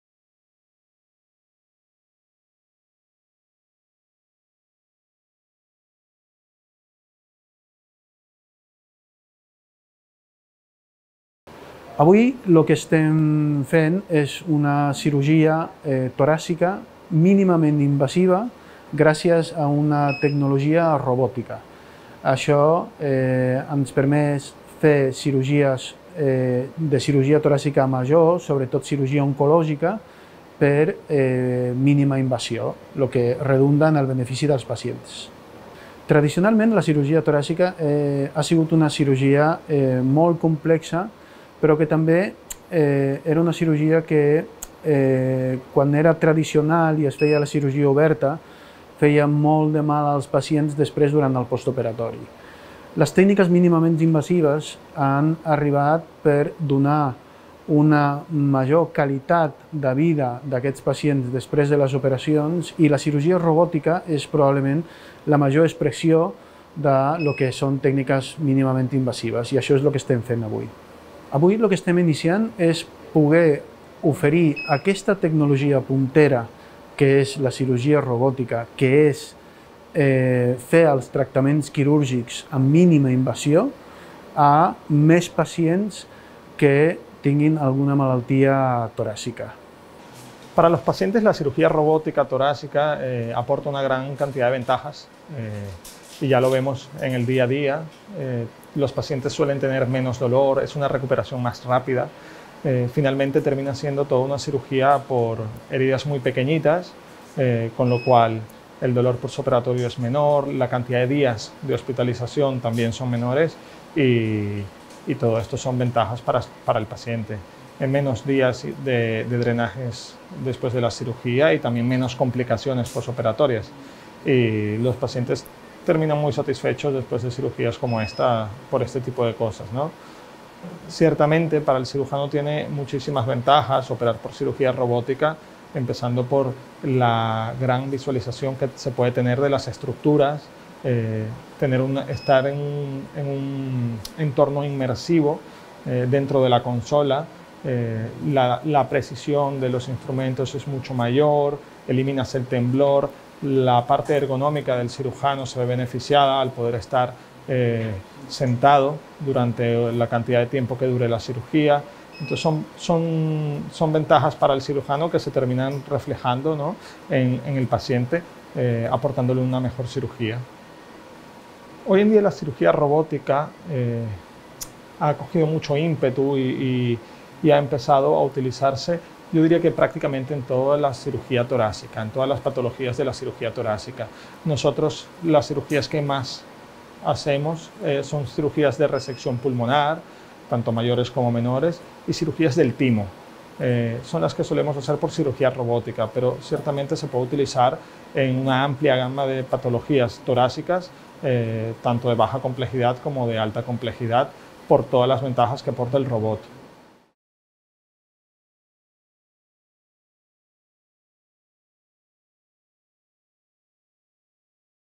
El Hospital Quirónsalud Barcelona realiza su primera cirugía torácica robótica
El Hospital Quirónsalud Barcelona acaba de realizar su primera intervención de cirugía torácica robótica a cargo del Dr. Albert Jáuregui, jefe del Instituto Jáuregui, y del Dr. Fernando Ascanio, cirujano torácico especialista en cirugía robótica del mismo hospital, quienes se han encargado de realizar una resección quirúrgica de una neoplasia pulmonar, que abre la puerta al uso de este abordaje en este tipo de intervenciones.